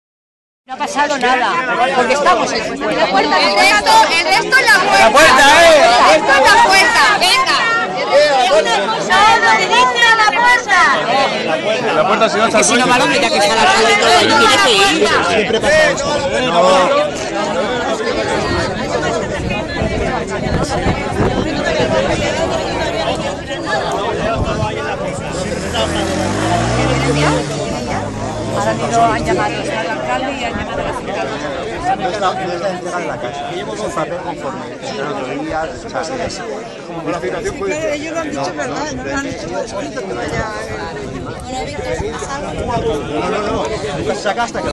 2Desnonament a Montornès Part 5 de 6
El dia 9 d'Abril de 2013 , després de pasar-nos tot el mati davant la porta d'en Carlos Rodriguez  per tal d'evitar el seu desnonament. L'acció la va organitzar la Pah Montornès , però hi vam asistir-hi varies PAH del Vallès a donar suport. Moltes gràcies a tots els que hi vau asistir. Al final amb la pressió pupolar s'ha aconseguit aturar fins el més de maig. I ens han assegurat que el proper cop que vinguin , entrarán si o si. Els hi demostrarem un altre cop que la organització i la lluita diaria es la clau per aconseguir els nostres objectius.
Ni una família més al carrer!!!
SI SE PUEDE!!!